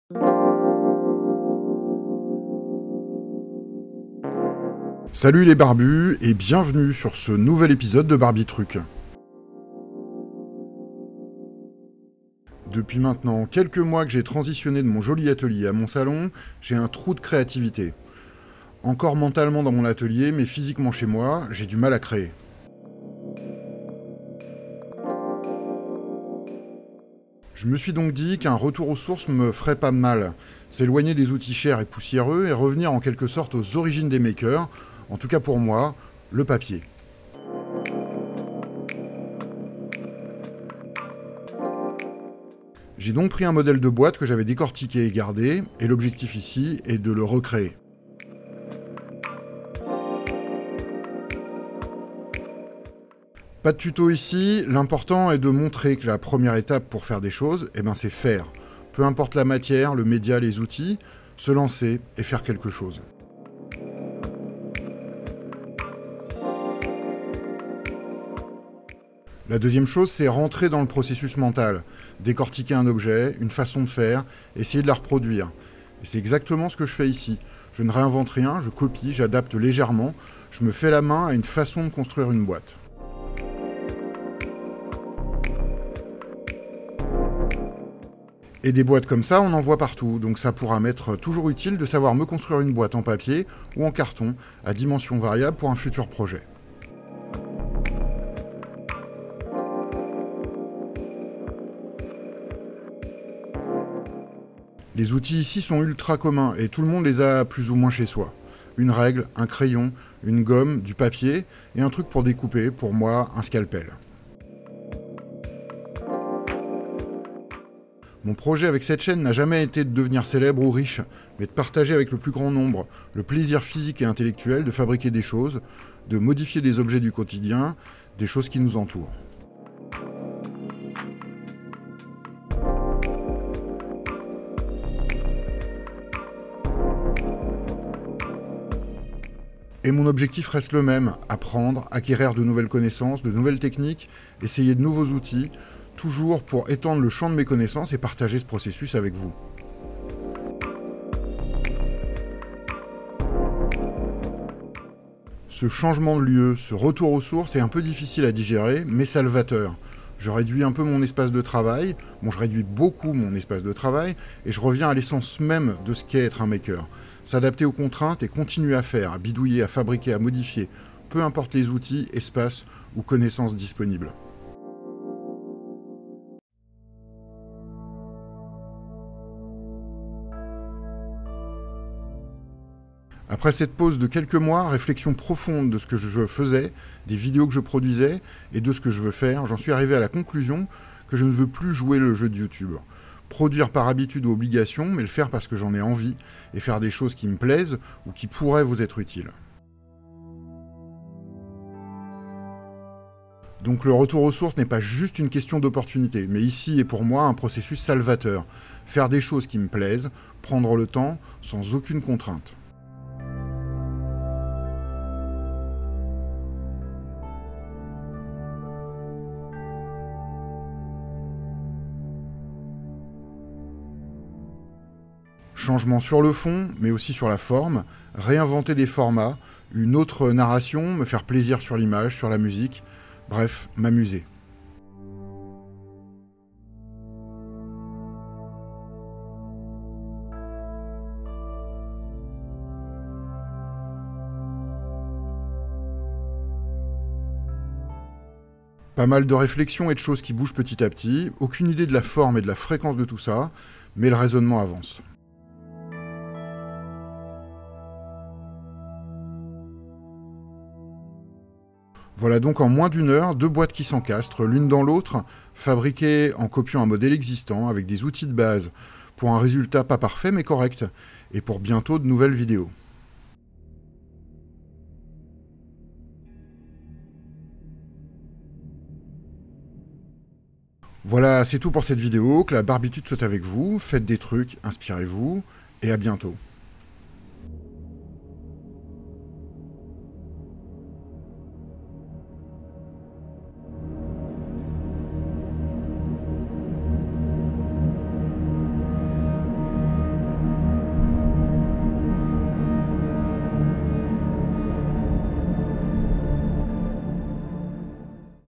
le papier, retour aux sources des makers - barbiTruc
un épisode de transition, où je me remets à faire, peu importe le média et ici je remets les mains sur du papier, un matériau pas cher, abondant, que tout le monde à chez soi et avec des outils basiques

dans pas longtemps, de nouvelles vidéos...
Patience donc